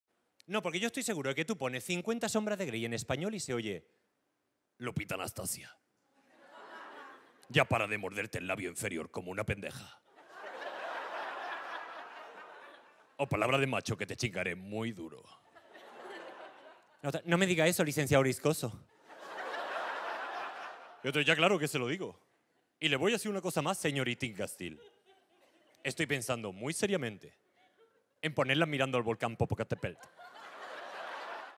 Doblaje Latino | Carlos Del Pozo | Comedy Central España
Carlos Del Pozo tiene una representación muy clara sobre cómo sería 50 Sombras De Grey si estuviese doblada al español latino.